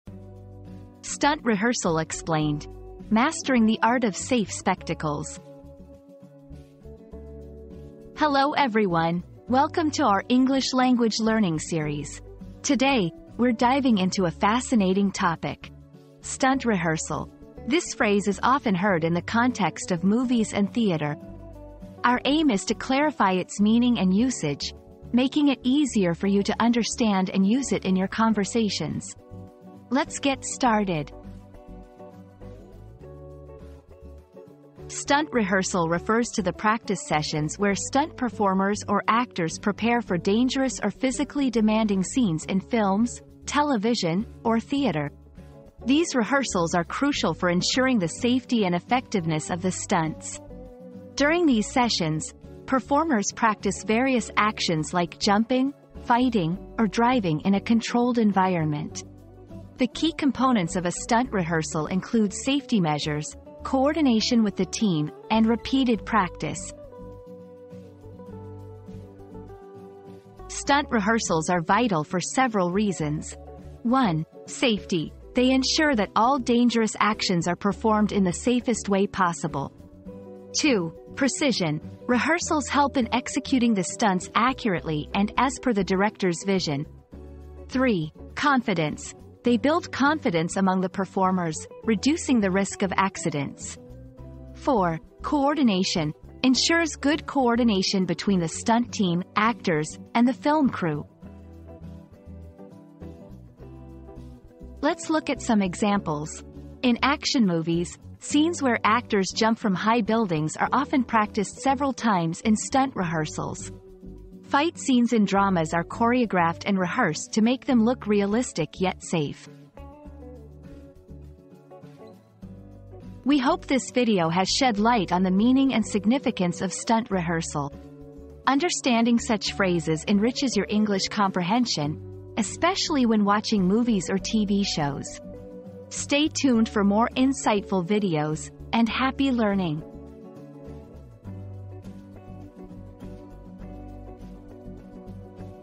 Stunt Rehearsal Explained: Mastering the Art of Safe Spectacles
Mastering the Art of Safe Spectacles: Stunt Rehearsal Explained • Discover the secrets behind mastering the art of safe spectacles in this captivating video. Join us as we delve into the world of stunt rehearsal and explore the meticulous preparation required to create jaw-dropping action sequences while ensuring the safety of the performers. Get ready for an adrenaline-pumping journey behind the scenes!

00:00 • Introduction - Stunt Rehearsal Explained: Mastering the Art of Safe Spectacles
00:34 • What Does 'Stunt Rehearsal' Mean?
01:09 • Why is Stunt Rehearsal Important?
01:44 • Real-World Examples of Stunt Rehearsal